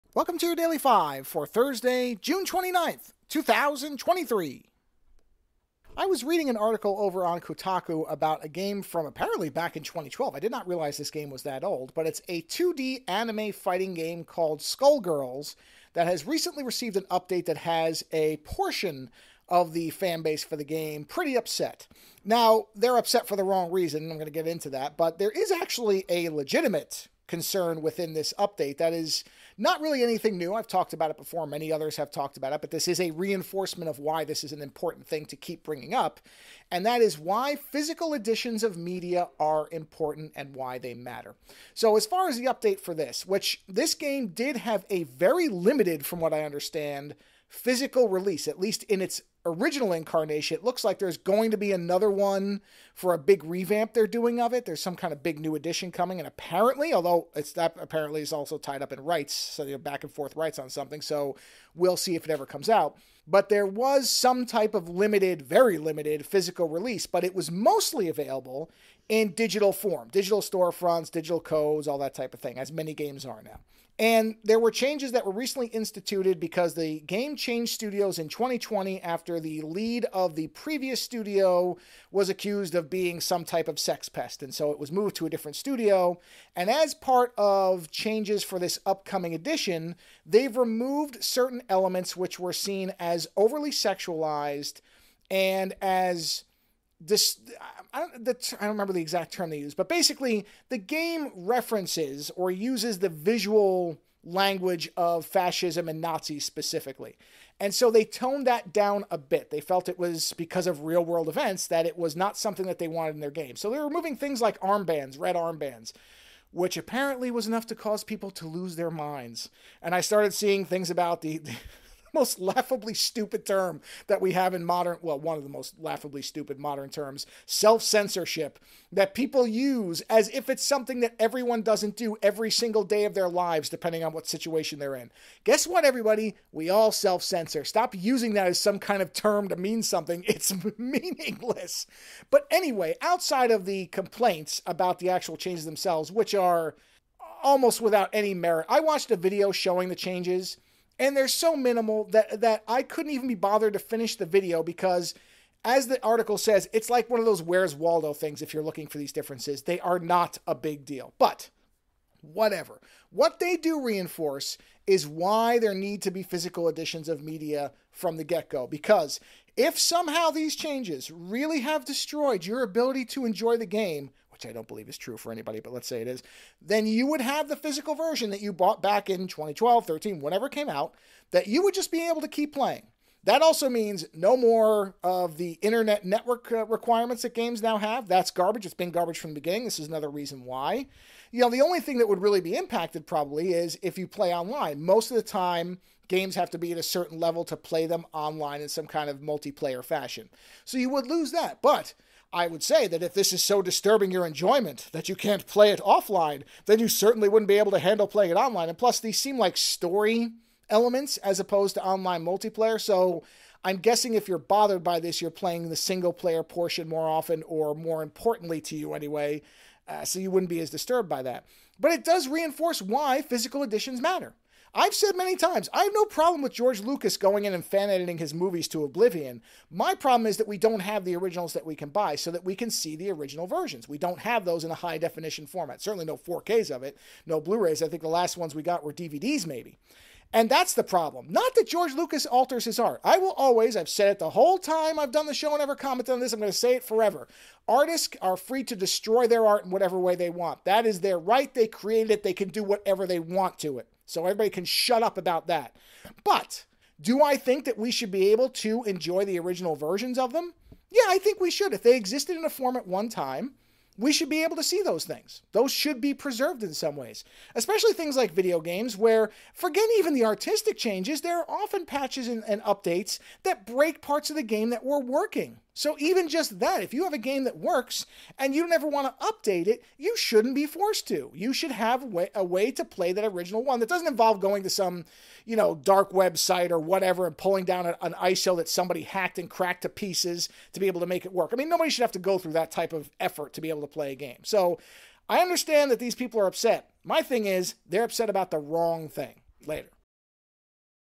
Ire Misfire
Today on the 5: The current studio behind the 2D fighting game Skullgirls recently issued an update to the game that a portion of the fanbase got upset over. Reading through the changes and reactions, it seems like most of the anger is focused on the wrong aspect of the story.